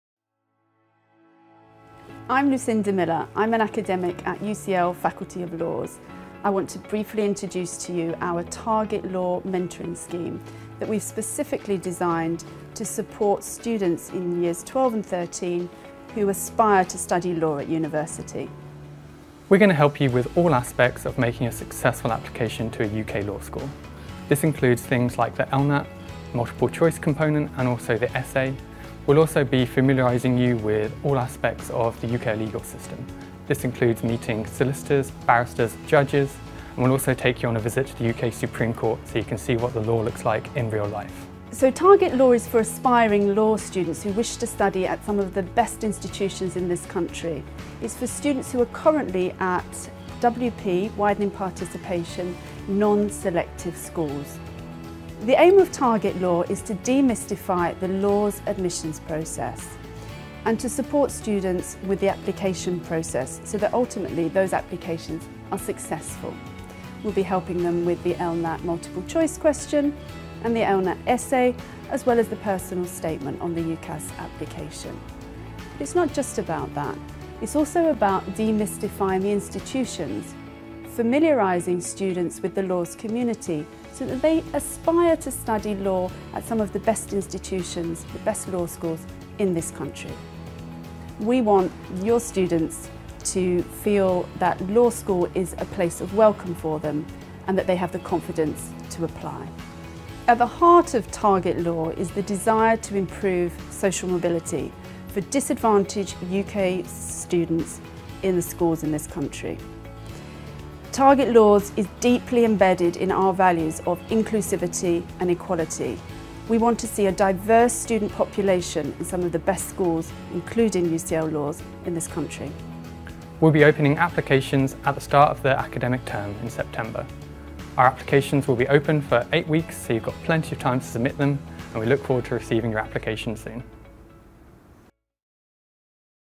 Target Law: UCL Laws' widening participation scheme
The Target Law Mentoring Scheme is one of our Widening Participation schemes at UCL Laws. The two year long programme is aimed at Year 12 students from disadvantaged groups in non-selective state schools with the aspiration to enter a top law school.